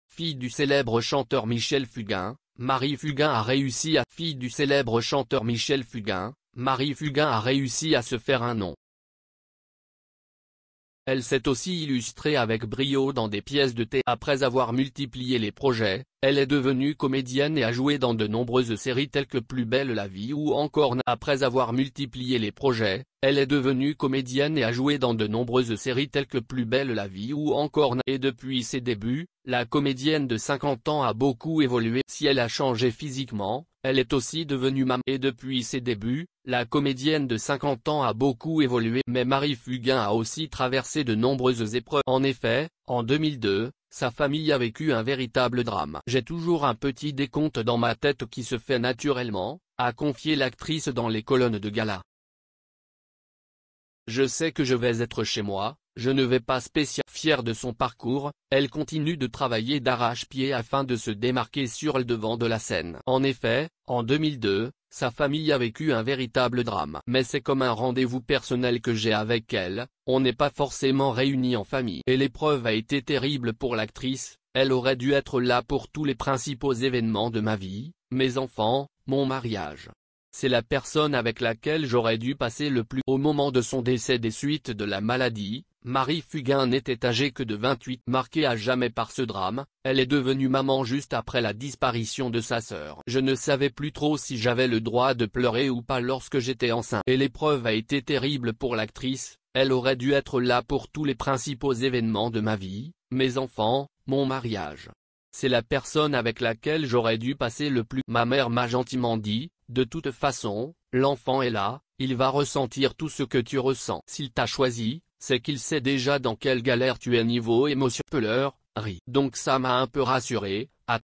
Marie Fugain a 50 ans : son évolution physique en images
Marie Fugain a 50 ans : son évolution physique en images Fille du célèbre chanteur Michel Fugain, Marie Fugain a réussi à se faire un nom. Après avoir multiplié les projets, elle est devenue comédienne et a joué dans de nombreuses séries telles que Plus belle la vie ou encore Navarro. Elle s’est aussi illustrée avec brio dans des pièces de théâtre. Fière de son parcours, elle continue de travailler d’arrache-pied afin de se démarquer sur le devant de la scène. Et depuis ses débuts, la comédienne de 50 ans a beaucoup évolué au fil des ans. Si elle a changé physiquement, elle est aussi devenue maman de deux garçons, Elliot en 2003 et Sam en 2006, fruits de son union passé avec le chanteur québécois Richard Charest. Mais Marie Fugain a aussi traversé de nombreuses épreuves dans sa vie. En effet, en 2002, sa famille a vécu un véritable drame. Sa petite sœur Laurette est décédée des suites d’une leucémie foudroyante : Chaque année, à l’approche de cette date, je me mets dans ma grotte. J